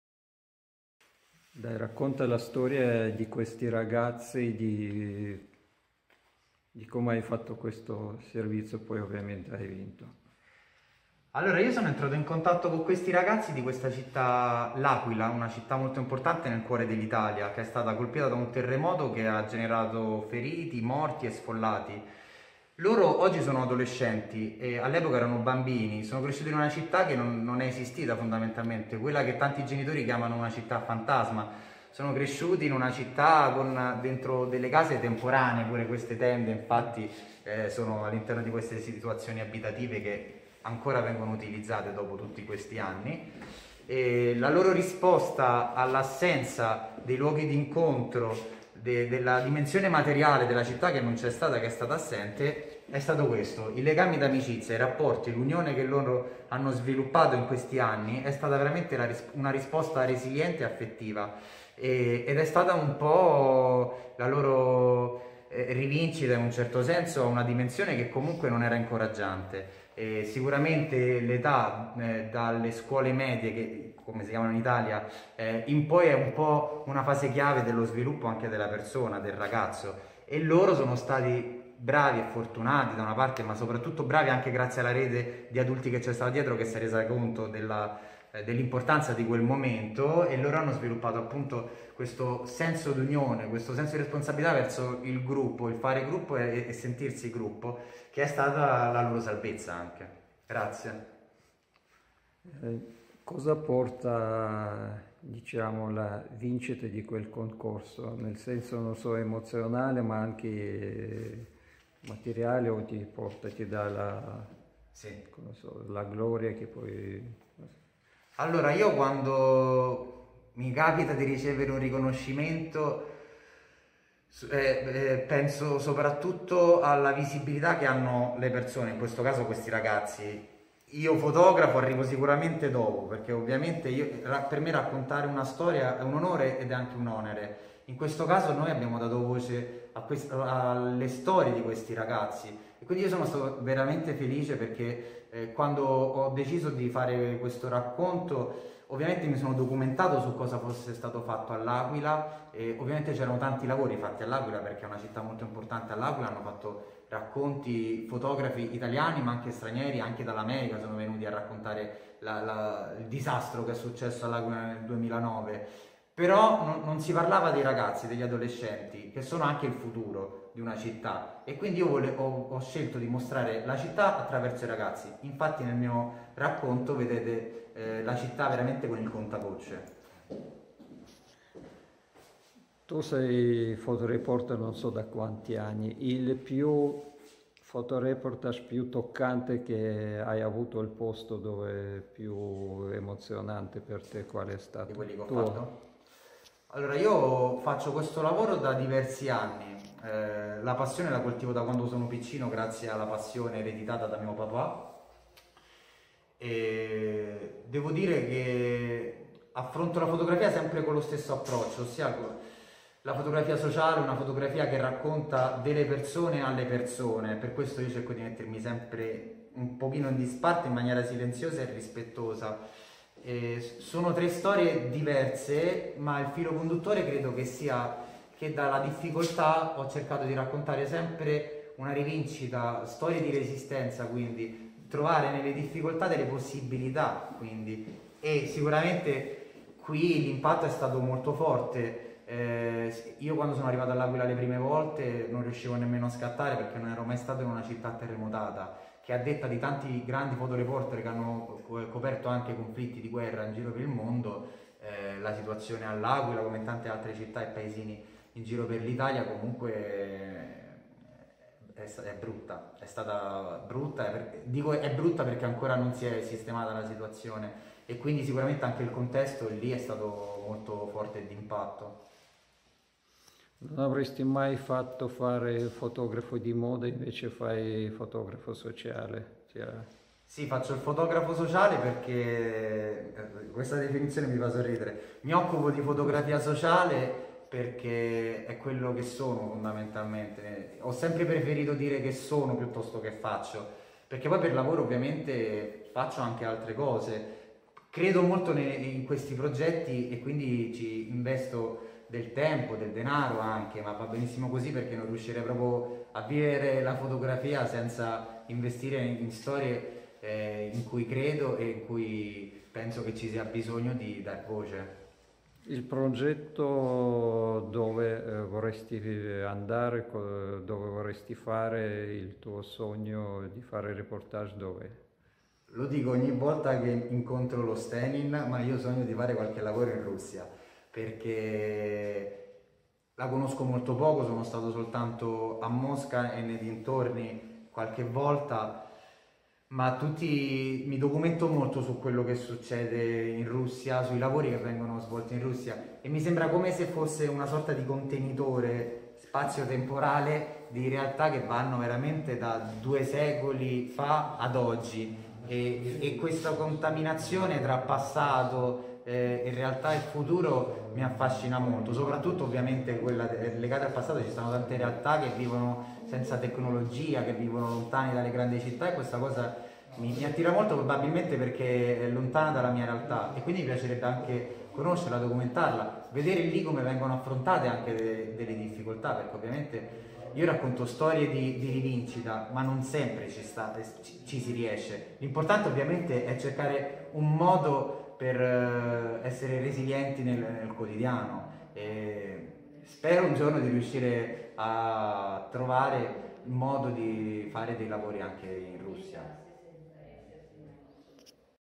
Danilo Garcia Di Meo - intervista
Intervista al giornalista Danilo Garcia Di Meo realizzata da Evgeny Utkin in occasione del Premio Internazionale di Fotogiornalismo Andrei Stenin, tenutosi a San Donato Milanese (MI).